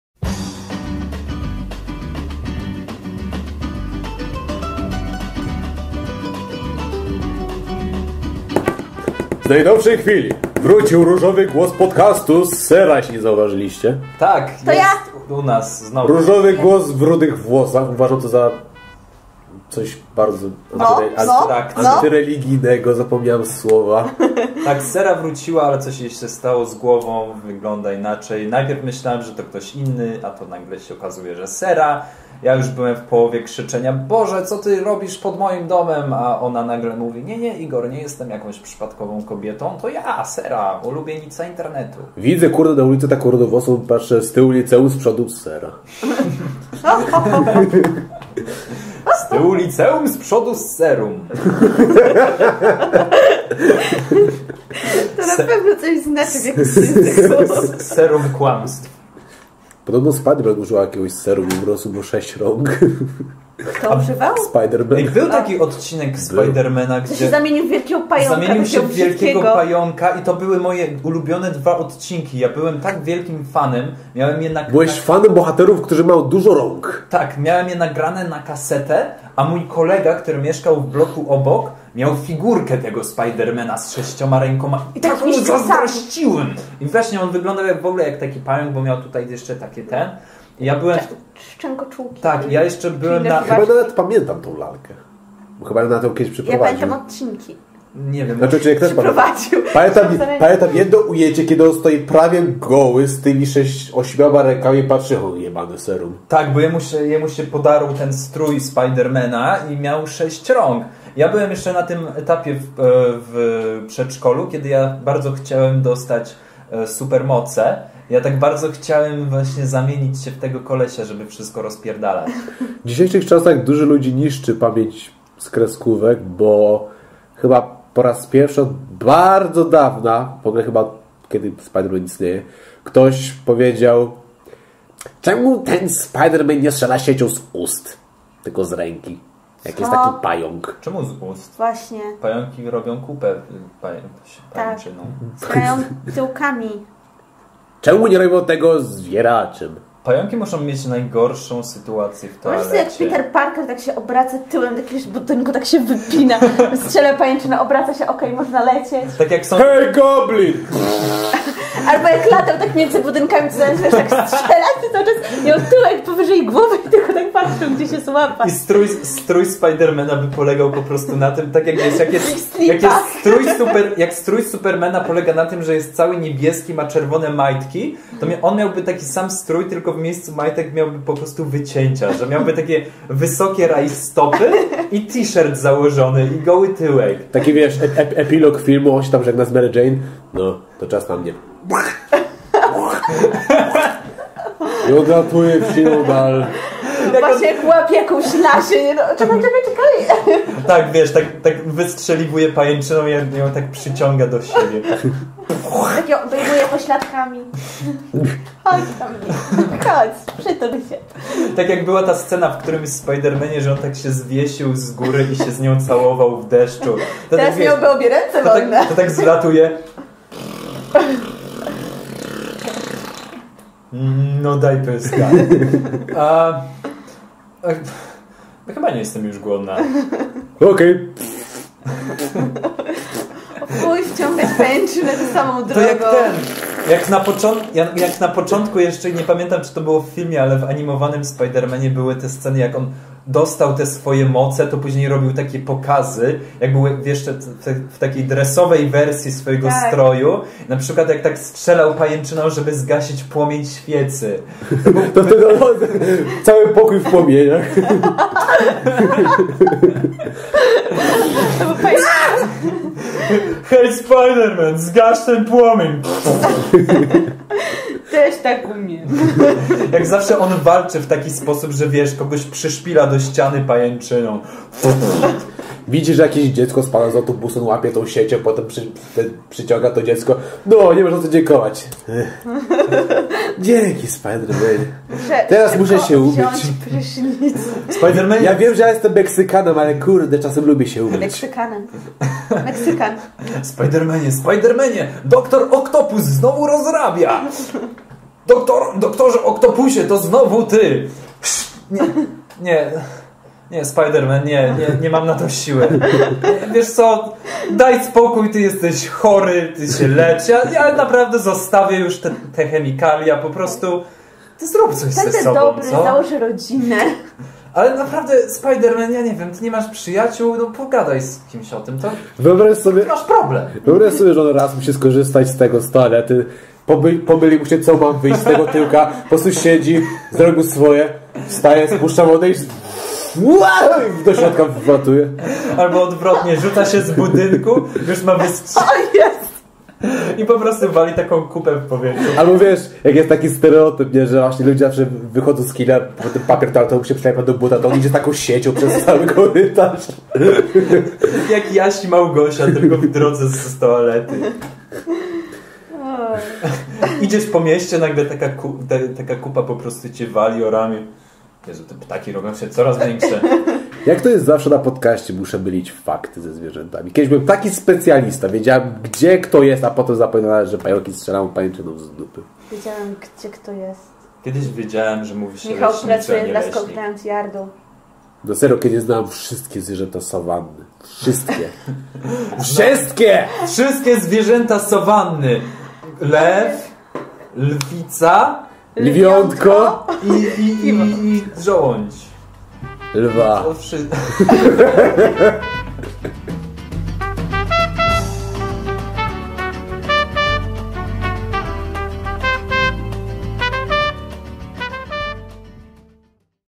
Besquitoo 16/05/2012: Nietojeże
Co znajdziecie w pełnej wersji:

- gdyby Spider-Man strzelał pajęczyną z tyłka
 - jeże prezentem na komunię
 - jaja wielbłąda
 - gdzie nie spojrzysz piłka nożna
 - Besquitoo to banda złodziei
 - nasze pomysły na filmy Pixara